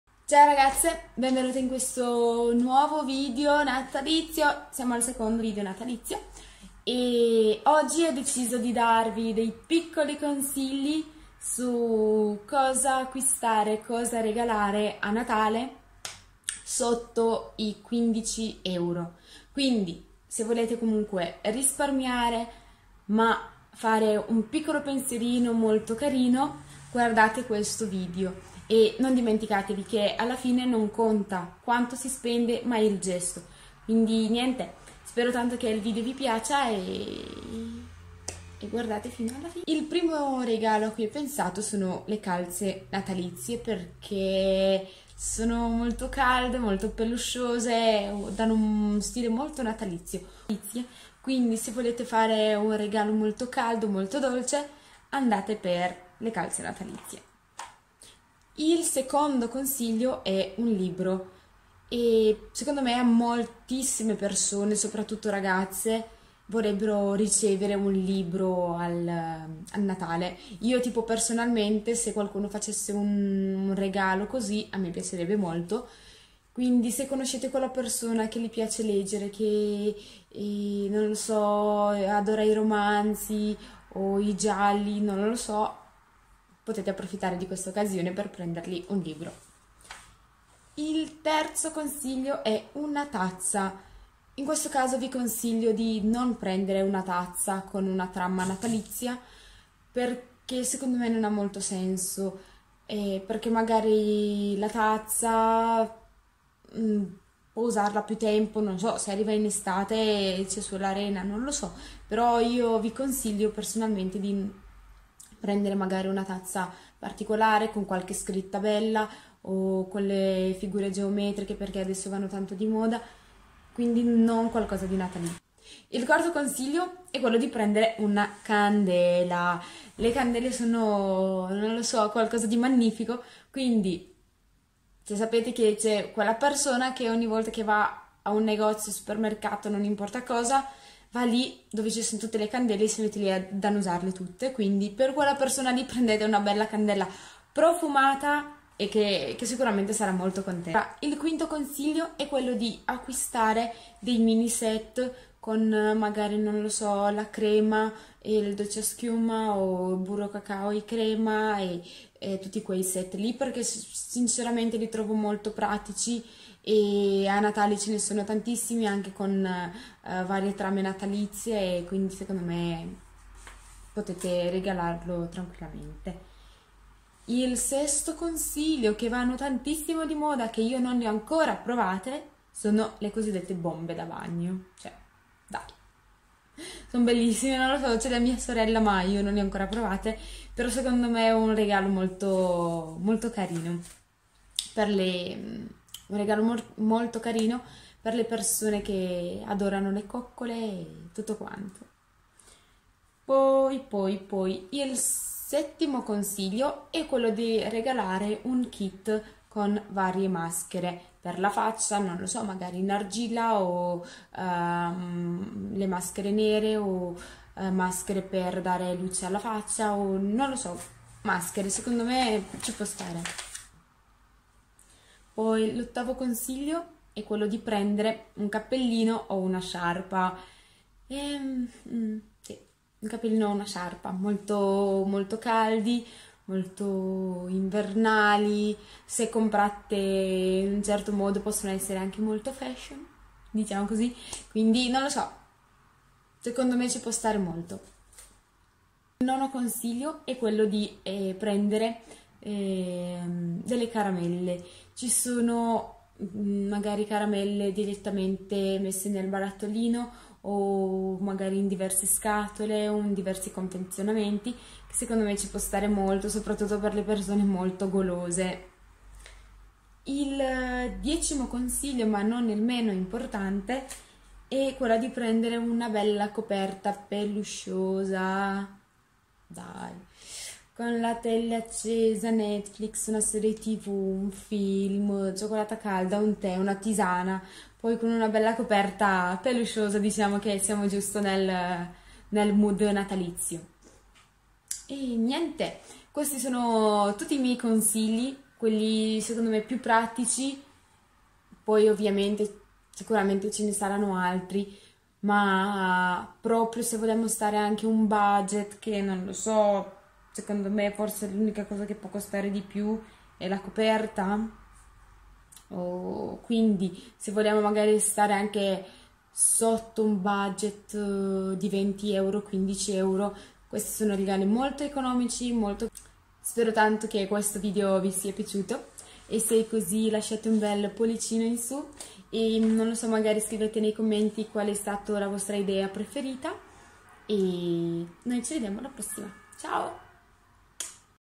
IDEE REGALO PER NATALE SOTTO I 15 €
Ciao ragazze ben venute in questo nuovo video…
Oggi vorrei darvi dei piccoli consigli su cosa regalare a Natale
Spero tanto che il video vi piace e noi ci vediamo con il prossimo video ….